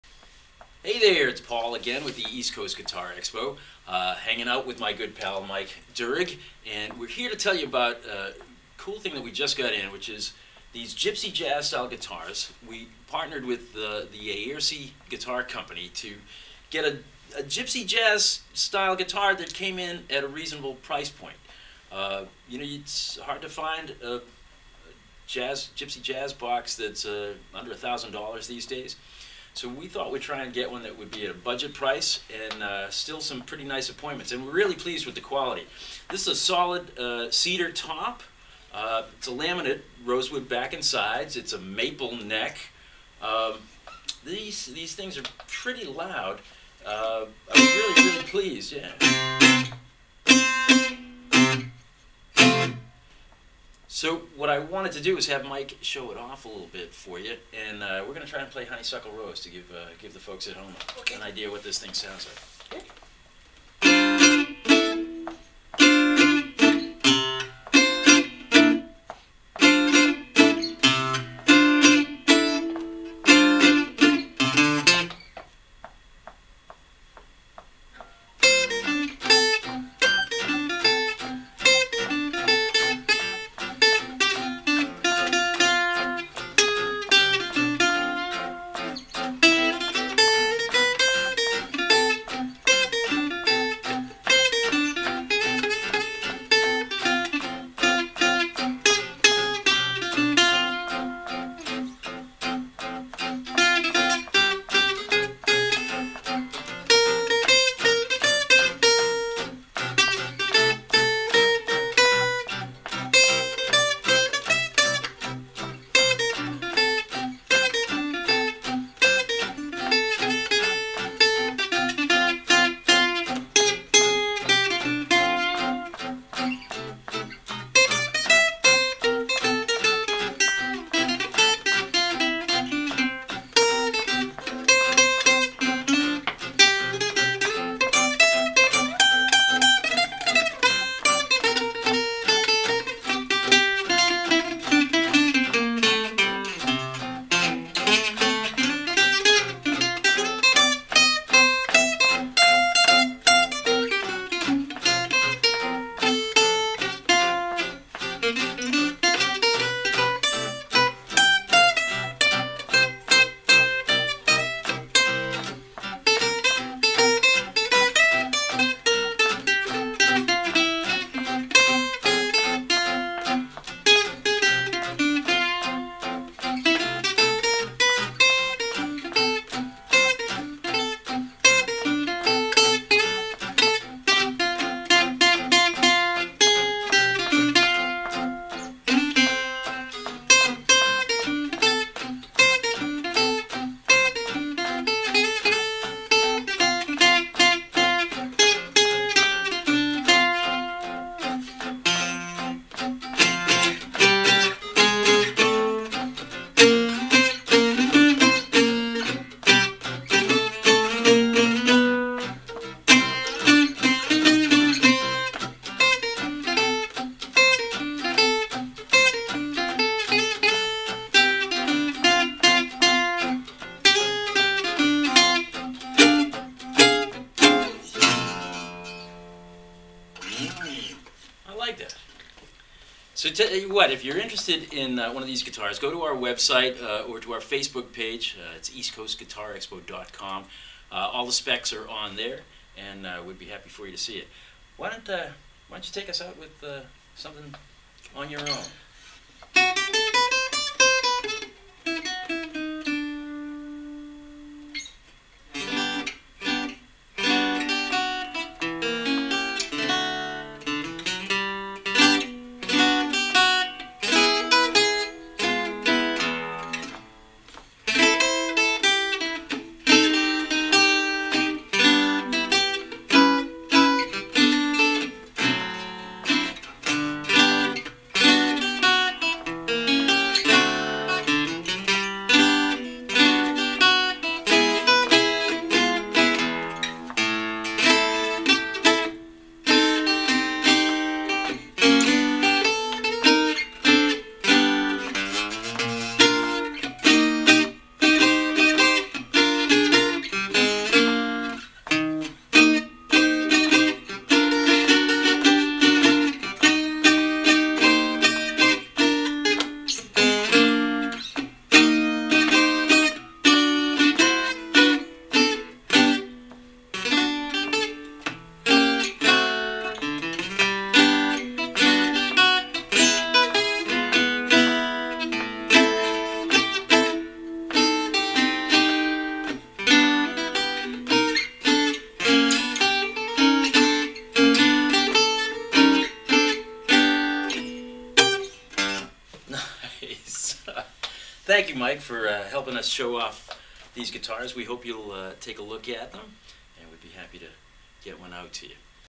AiersiGypsyModel
Mike Durig helps demonstrate the Gypsy Jazz models we just got in from the Aiersi Guitar Company. I'm playing an Aria that I had modified into a tenor-hybrid guitar.